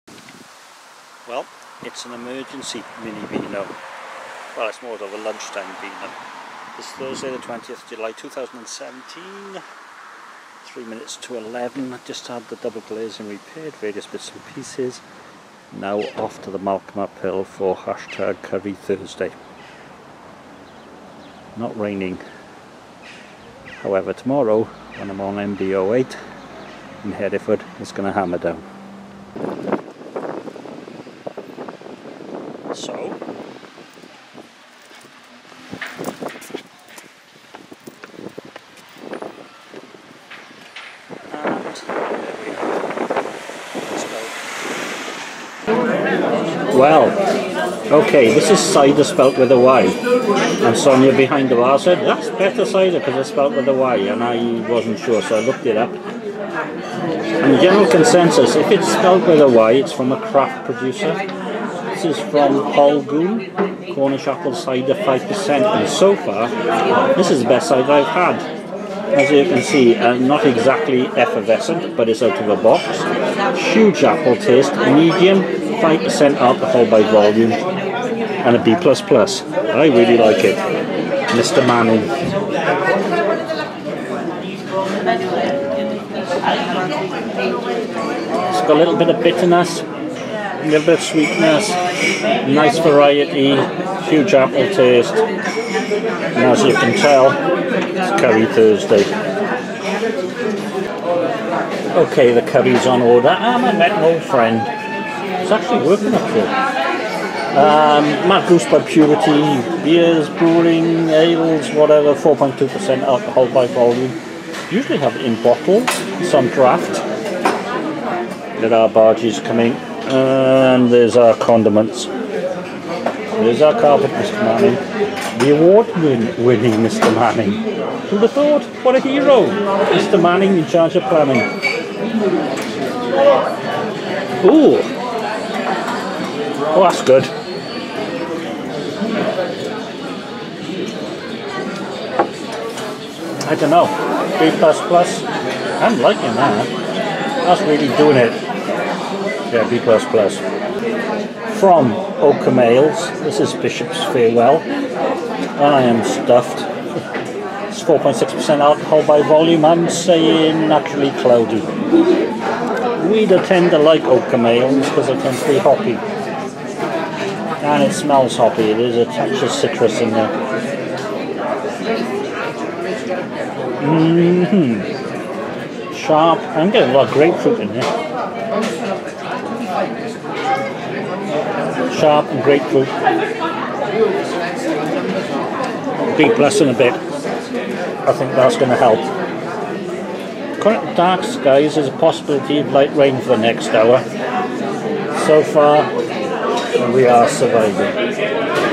Double Curry Thursday
A day off for Double Glazing maintenance then CurryThursday in the Malcolm Uphill. The Cider Festival is still on and there were some Fine Ales. Rather over did it with the Curry.